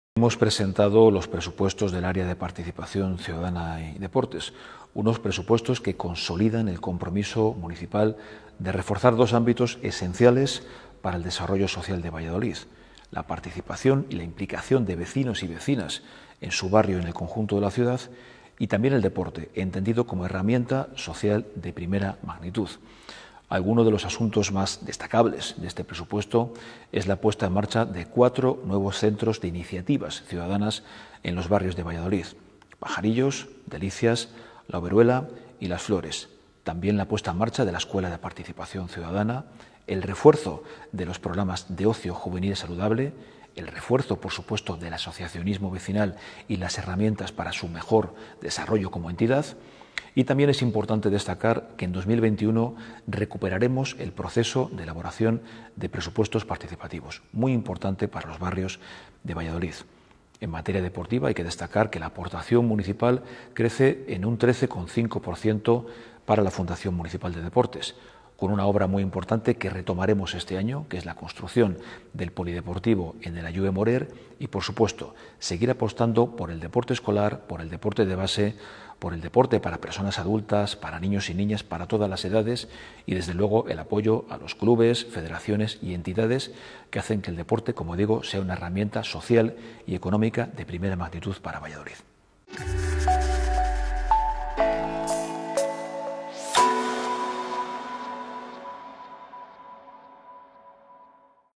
Alberto Bustos presenta el presupuesto para 2021 de Participación Ciudadana y Deportes
Alberto Bustos presenta unos presupuestos para impulsar el desarrollo social de Valladolid:
- Nuevos Centros de Iniciativas Ciudadanas.
- Escuela de Participación.
- Regreso de los Presupuestos Participativos.
- Apoyo al deporte, desde la base hasta la élite.